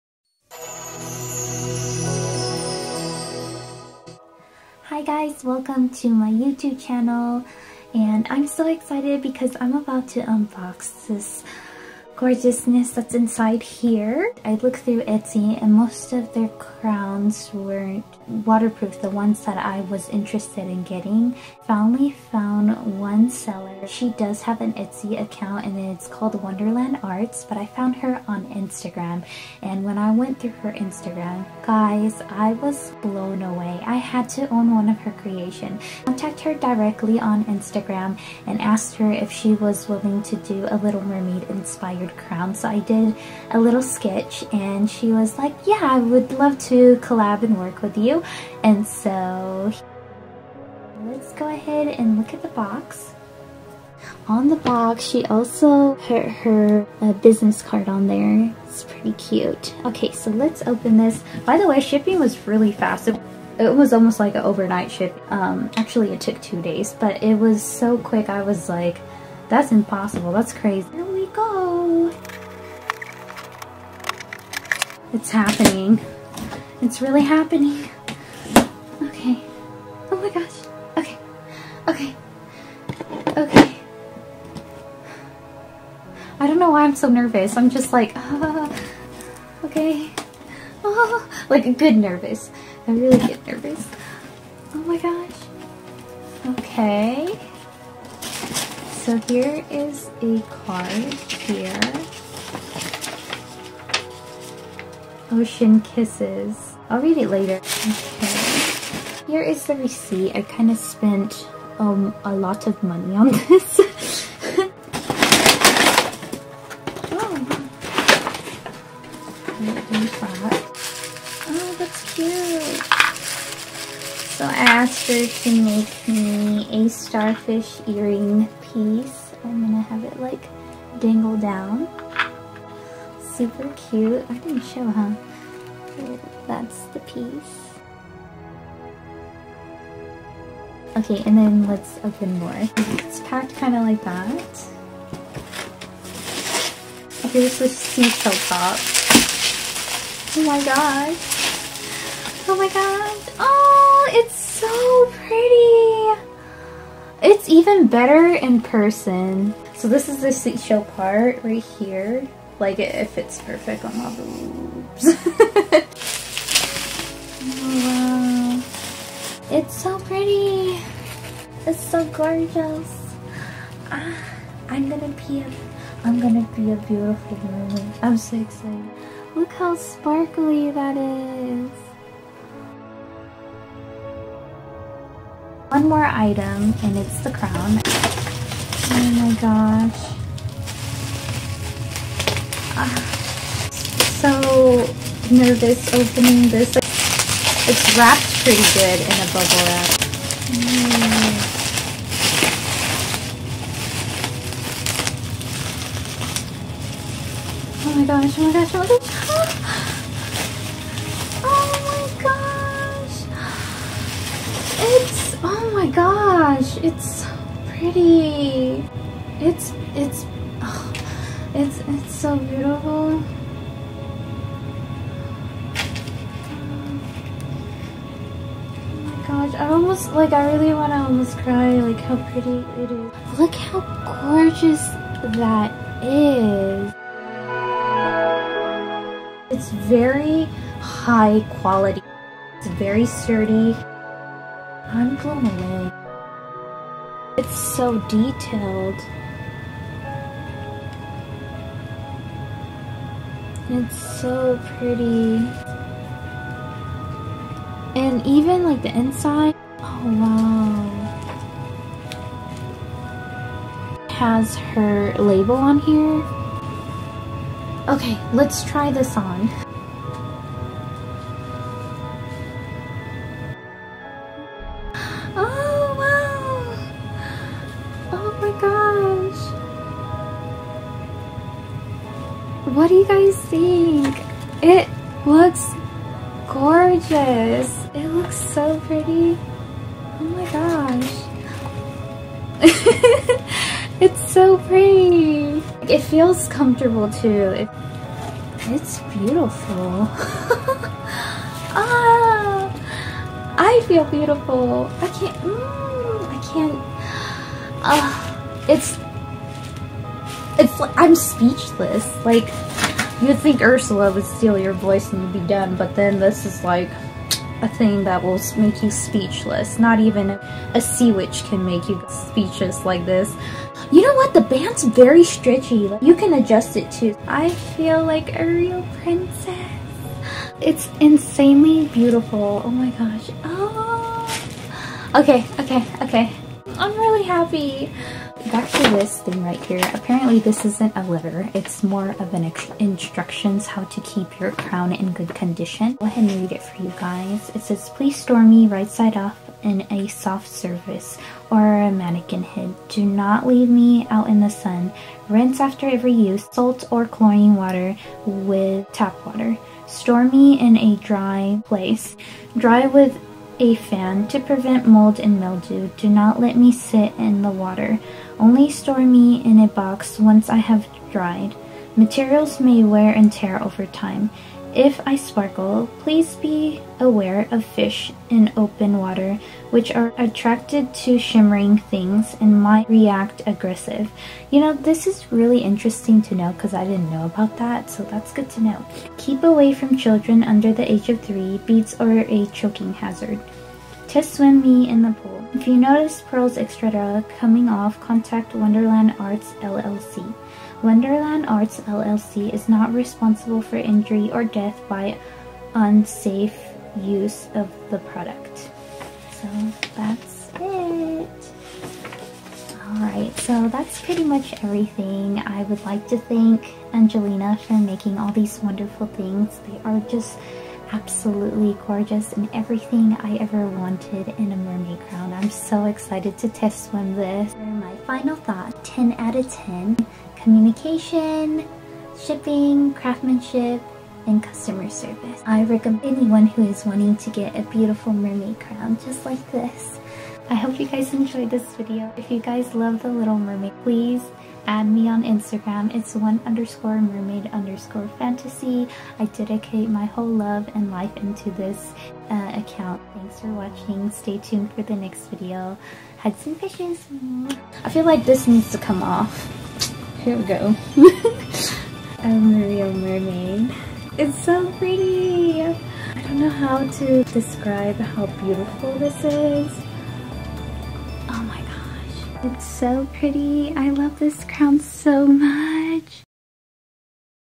UNBOXING My New Mermaid Crown By Wunderland Arts !!!
Hope you guys enjoy this unboxing video
*** I apologize for the AC that was going off at the time of this blog and the poor quality of the audio.

link to etsy Shop for WunderlandArts
link to instagram: Wunderland_Arts